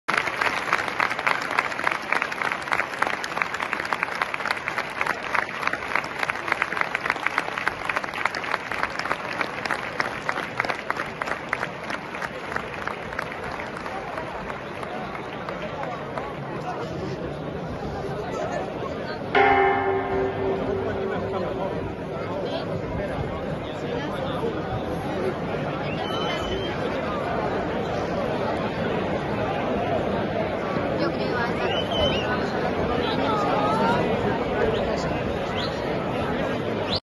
Carnaval ciudad rodrigo minuto silencio
El Ayuntamiento de Ciudad Rodrigo condenó de forma unánime y “enérgicamente y sin paliativos” el fallecimiento de un joven de 17 años tras una pelea en la madrugada de este sábado en la calle Colada del pueblo, pero sigue con la fiesta sin inmutarse tras un minuto de silencio.